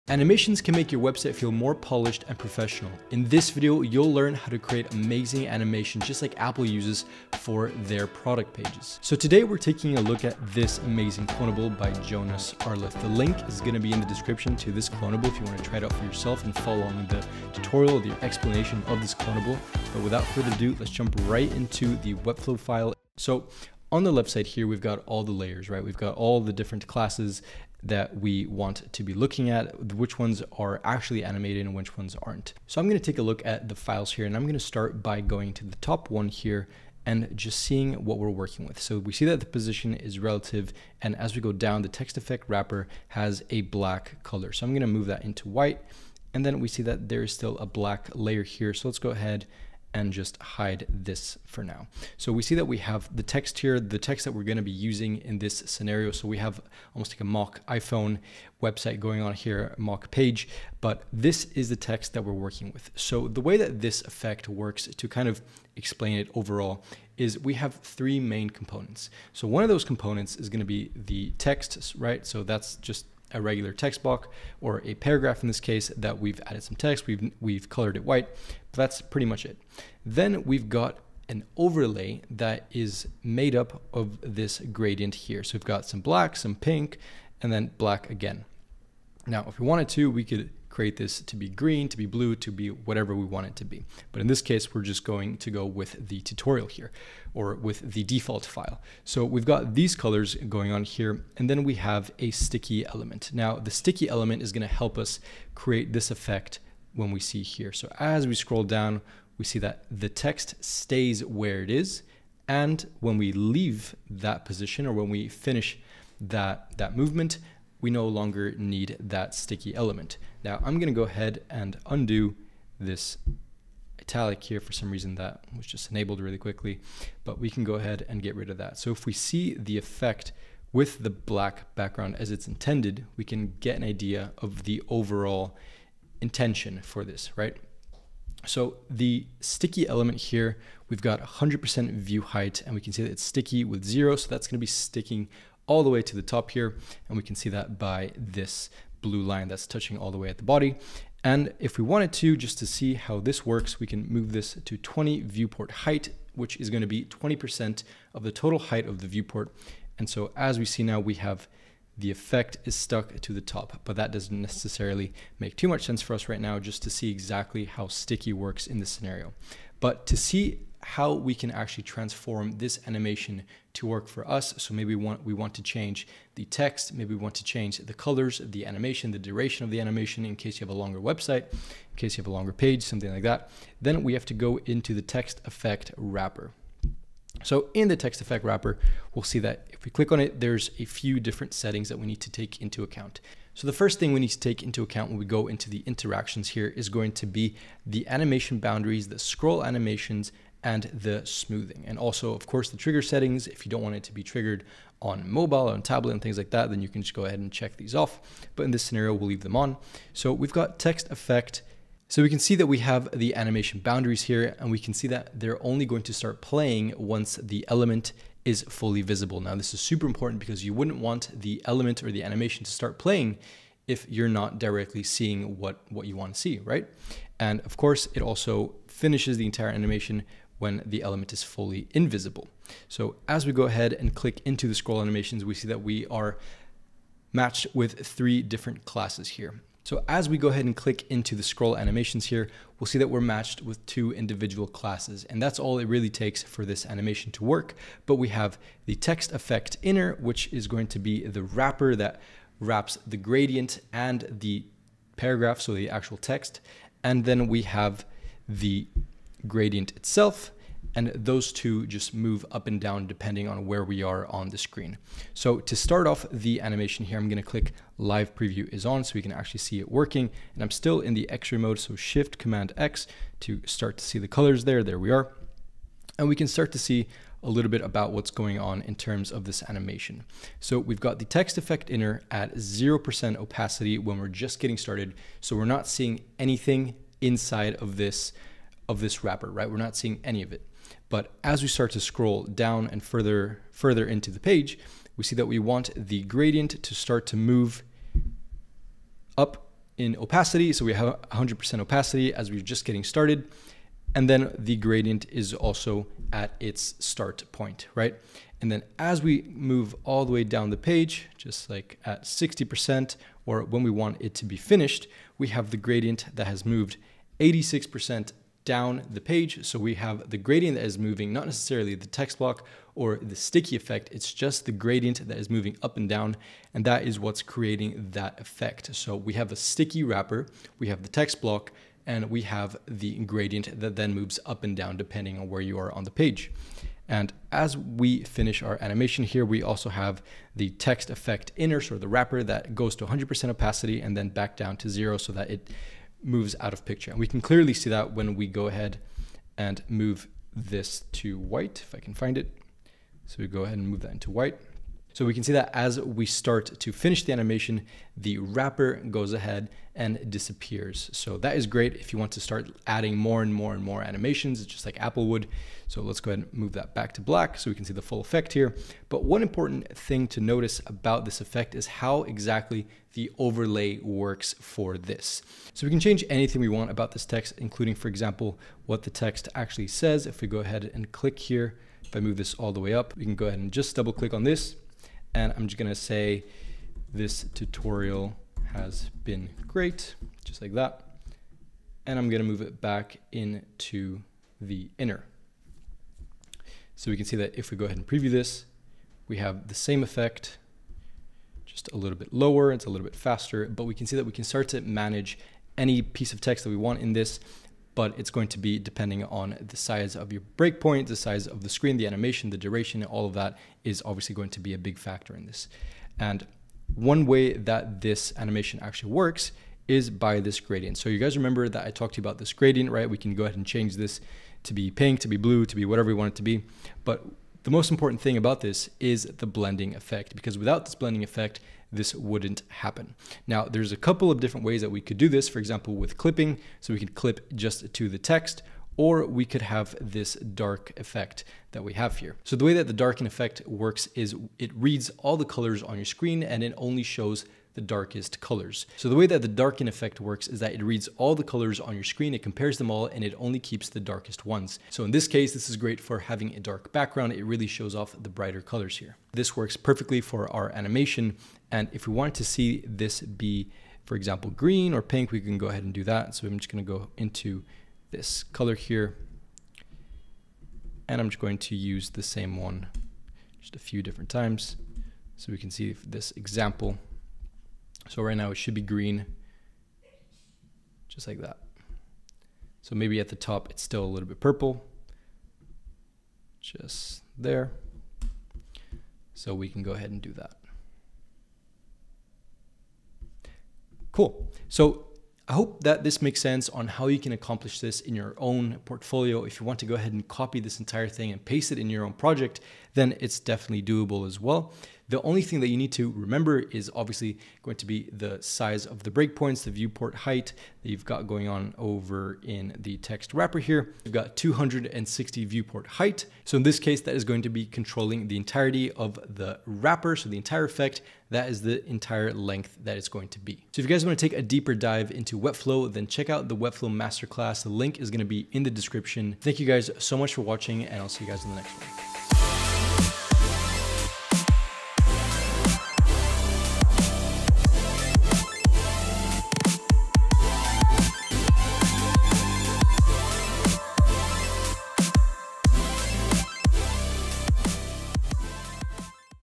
Apple Text Gradient Animation With Webflow Tutorial
Learn Webflow the FAST way with our full Webflow Masterclass course

Animations can make your website feel more polished and professional! In this video you’ll learn how to create this amazing animation JUST like the one Apple uses for their product pages. We’ll be using Jonas Arleth’s work who created the original cloneable we’ll be utilizing today.

📽️ CHAPTERS
00:00 - Intro
00:22 - Cloneable Breakdown
02:49 - Editing The Animation
06:09 - Clarifying Layers
07:07 - Blending Effect